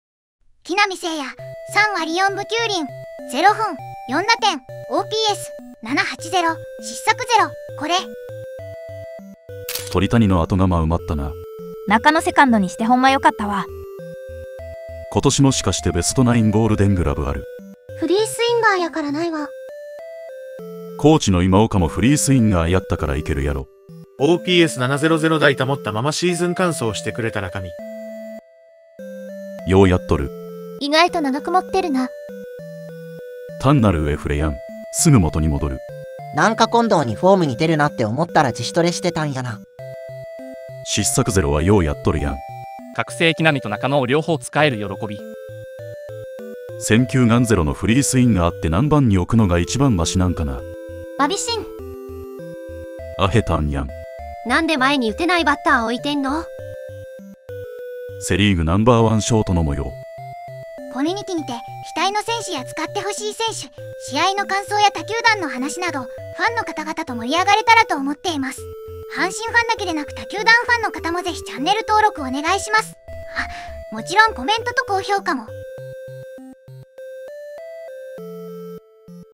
【最強ショート】木浪聖也 .349 0本 4打点OPS.789 失策0←これ【阪神タイガース/プロ野球/なんJ反応まとめ・ 2chスレ・5chスレまとめ/VOICEVOX】
⚾音声提供
VOICEVOX:ずんだもん
VOICEVOX:青山龍星
VOICEVOX:No.7
VOICEVOX:雨晴はう
VOICEVOX:剣崎雌雄
VOICEVOX:もち子さん
VOICEVOX:白上虎太郎
VOICEVOX:玄野武宏
VOICEVOX:春日部つむぎ
VOICEVOX:四国めたん